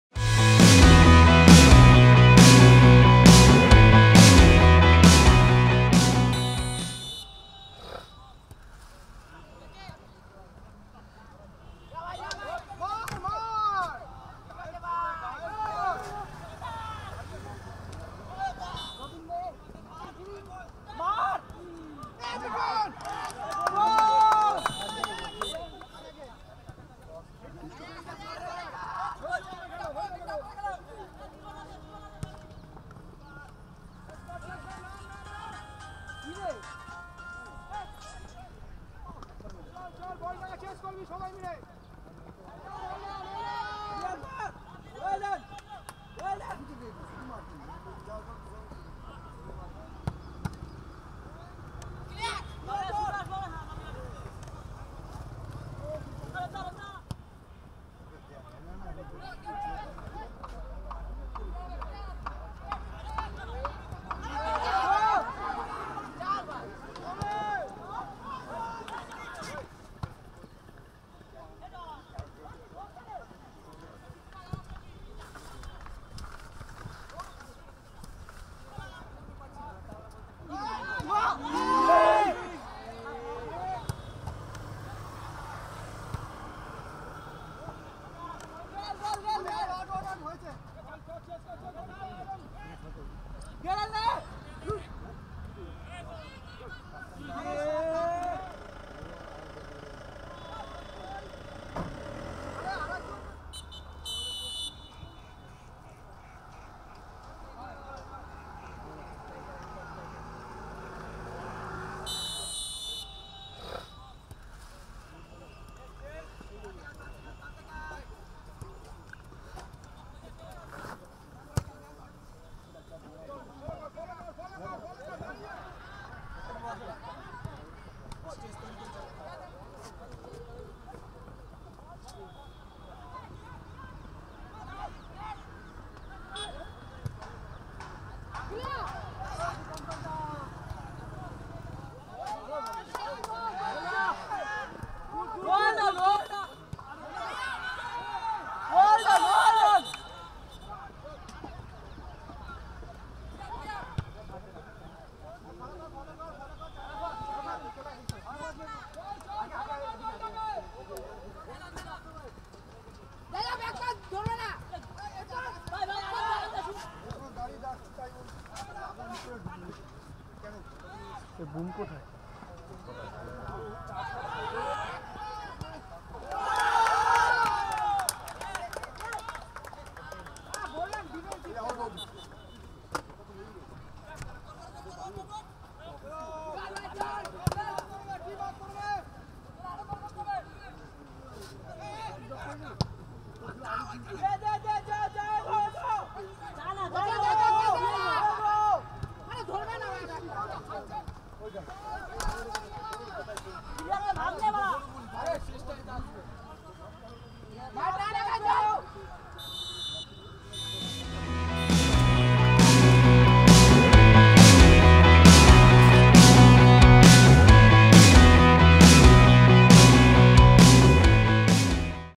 RFYS: Kolkata Sr. Boys - Halisahar Mallickbag H.S. vs Durganagar Bipin Behari Vidyapith Highlights
Watch the match highlights of RFYS: Kolkata Sr. Boys - Halisahar Mallickbag High School vs Durganagar Bipin Behari Vidyapith Academy match held on 22nd October, 2016.